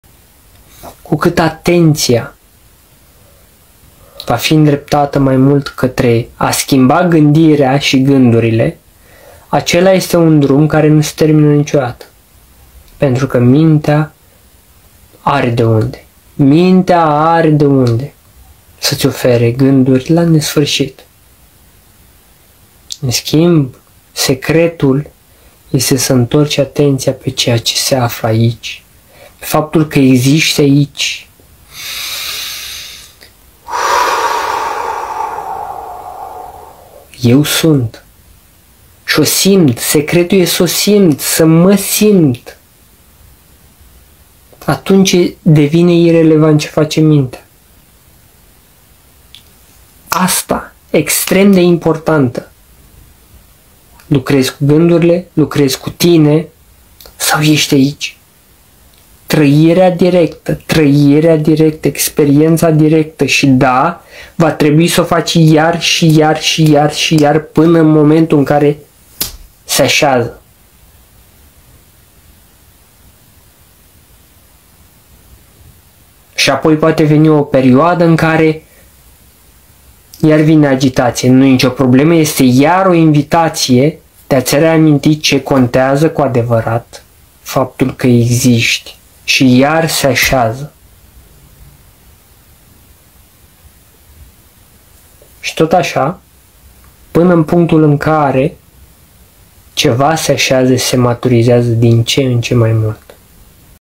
Trăirea directă - Marius Chirilă
Doar prin Trăire Directă poți descoperi Adevărul despre Tine şi despre Viață. Tot ce nu înseamnă Trăirea Ta Directă reprezintă doar hărți de diferite mărimi, unele mai detaliate, altele mai puțin, altele învechite sau altele total greşite. Dar orice hartă, oricum ar fi ea, cel mult poate doar indica teritoriul, Experiența Directă apare doar atunci când păşim Conştienți pe teritoriul Ființei Noastre.